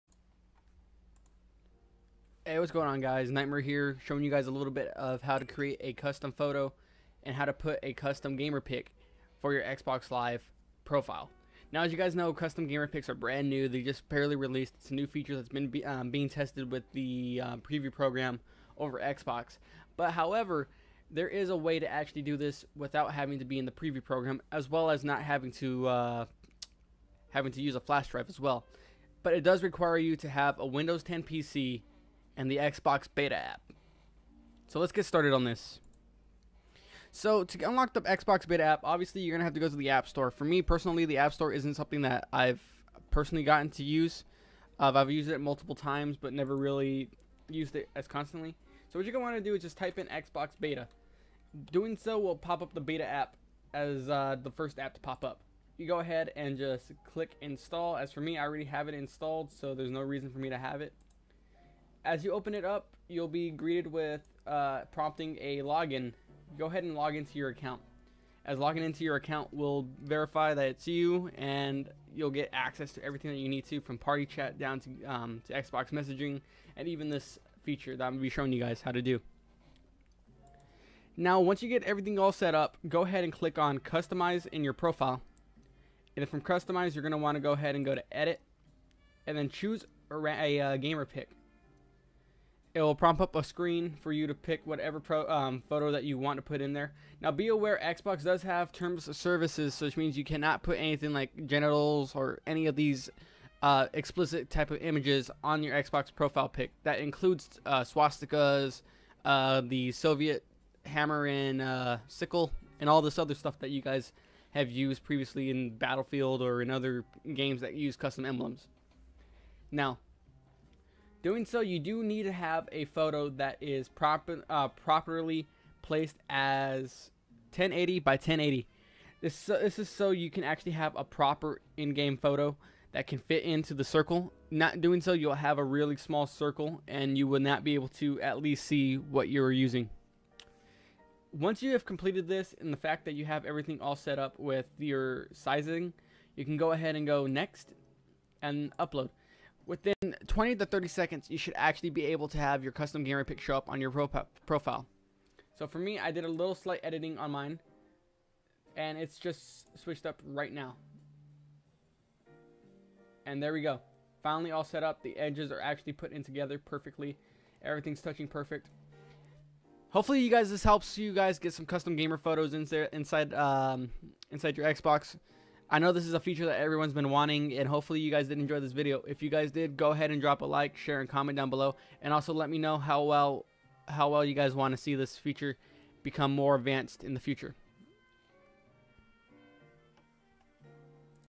Custom Xbox One GamerPic (Xbox App)
Here is how you can change your Xbox Avatar or Gamepic to a custom photo

Requirements:
Photo 1080x1080
Must be PNG or Jpeg
Windows 10 PC
Xbox Beta App